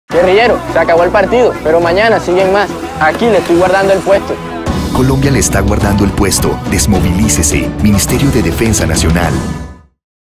El pescador dice
Guerrillero, no se pierda la fiesta más grande del fútbol, no lo piense más y desmovilícese que aquí le guardamos el puesto. Campaña para la desmovilización.

Recuerde utilizar el yoleguardoelpuesto.